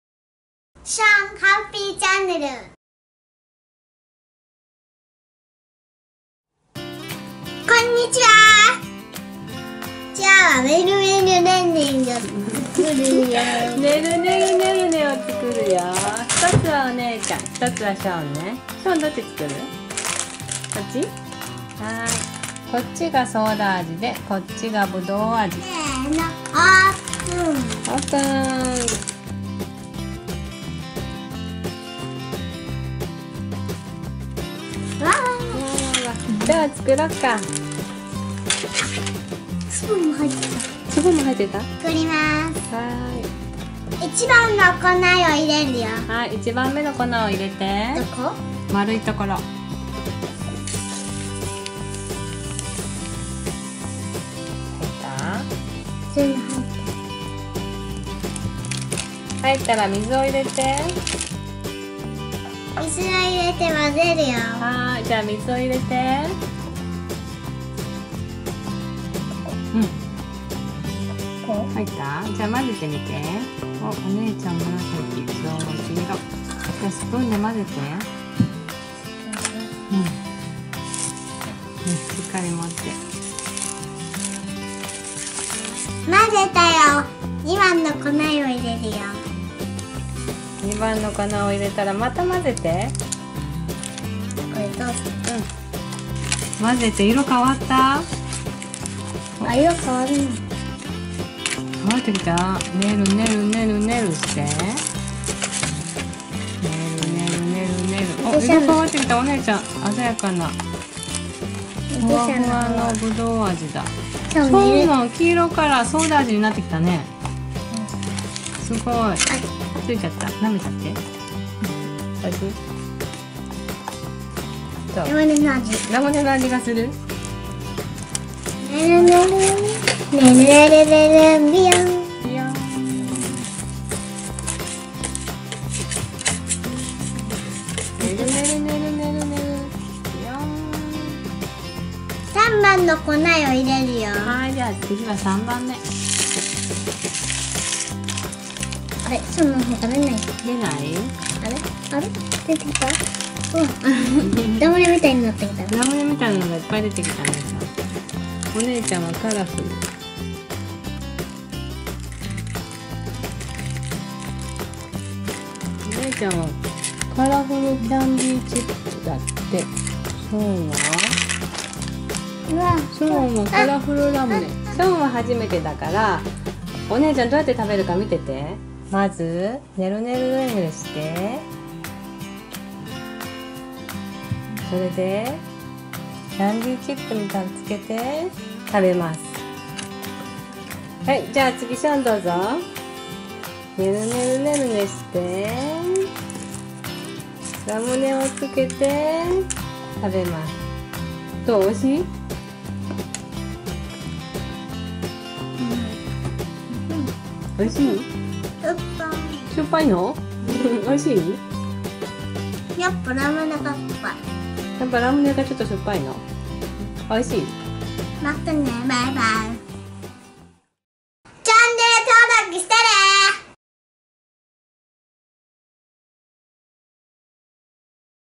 4歳になって初めて「ねるねるねるね」を作って食べてみたよ！
はじめて知育菓子の「ねるねるねるね」を作ってみたよ。ソーダ味もブドウ味もどちらも美味しかったよ！